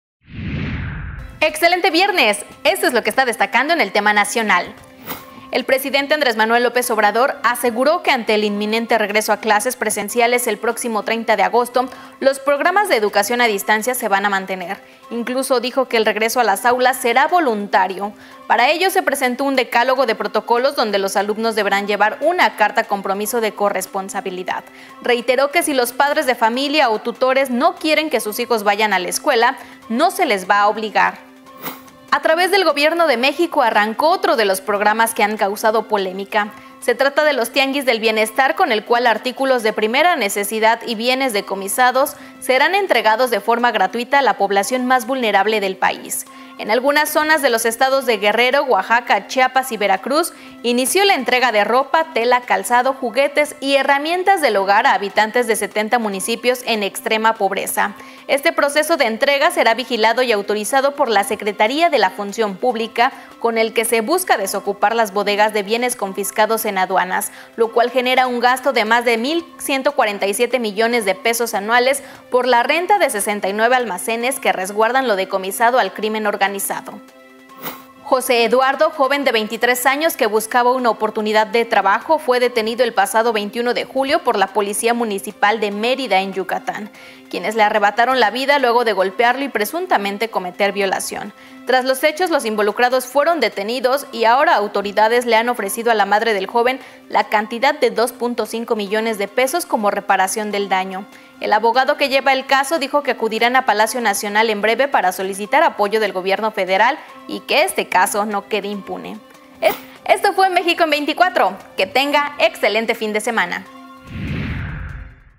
MÉXICO EN 24 | 13.08.2021
MÉXICO EN 24 | 13 DE AGOSTO DE 2021

ARRANCA PROGRAMA "TIANGUIS DEL BIENESTAR"

A través del Gobierno de México arrancó otro de los programas que han causado polemica, se trata de los Tianguis del Bienestar con el cual  artículos y bienes decomisados serán entregados de forma gratuita a la población más vulnerable.

Aquí lo que destaca en el tema nacional con Carol Osegueda.